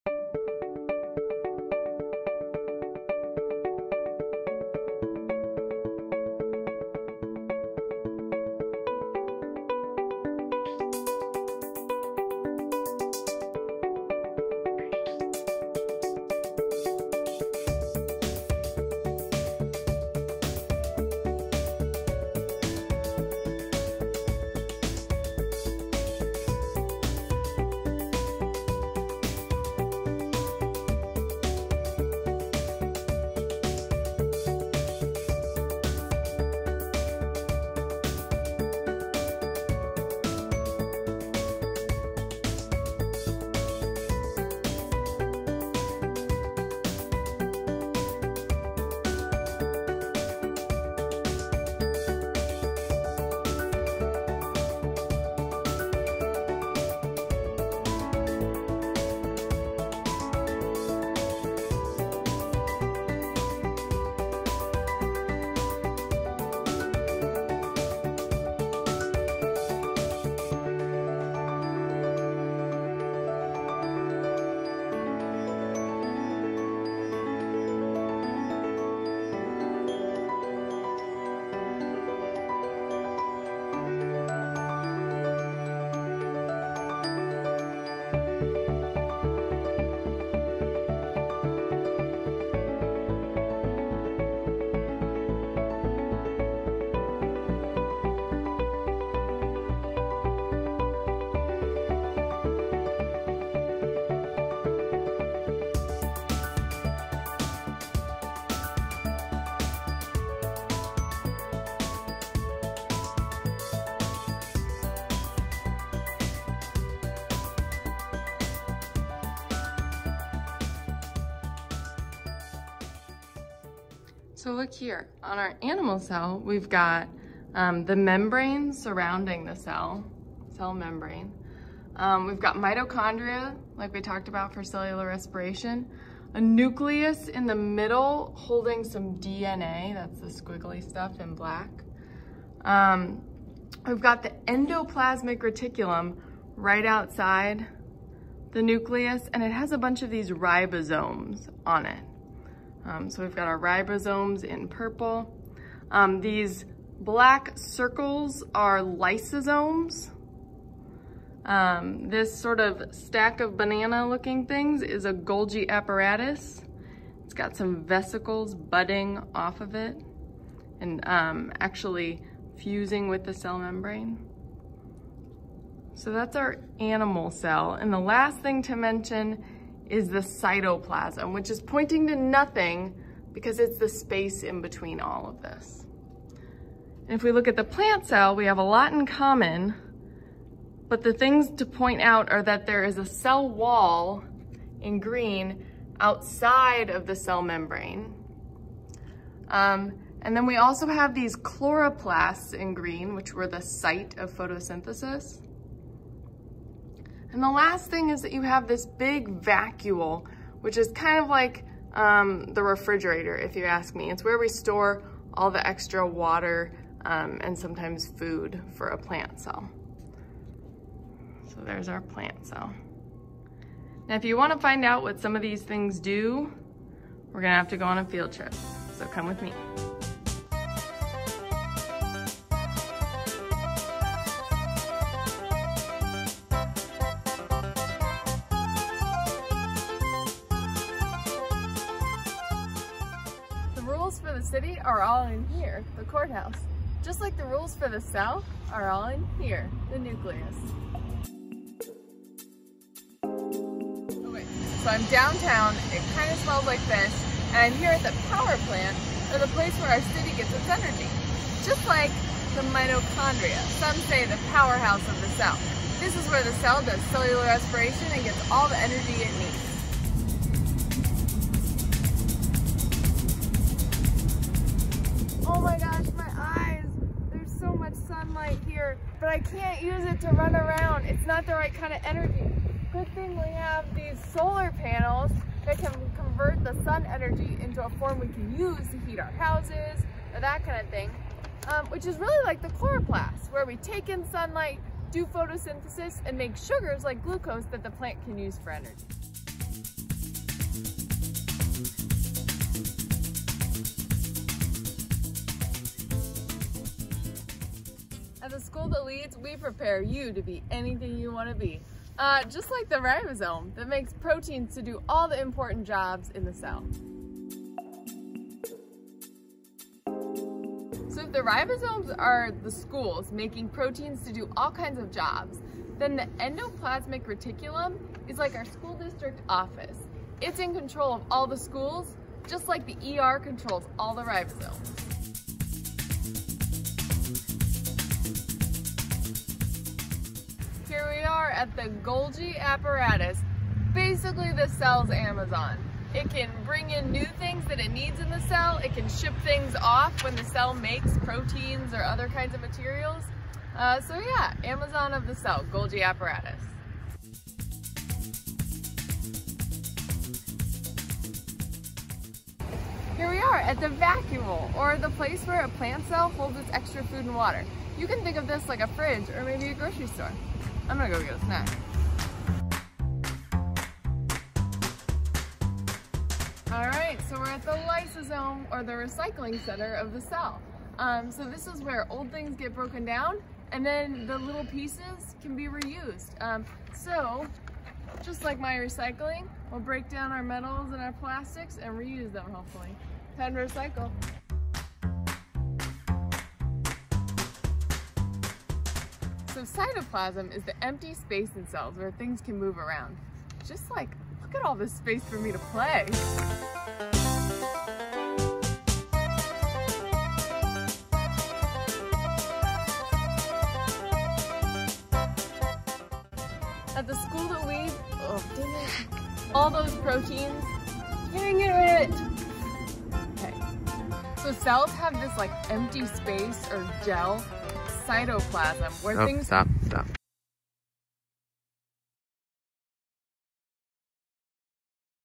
Cells Tour
Label the parts of animal & plant cells and learn about the functions of each cell organelle while taking a tour of Iowa City!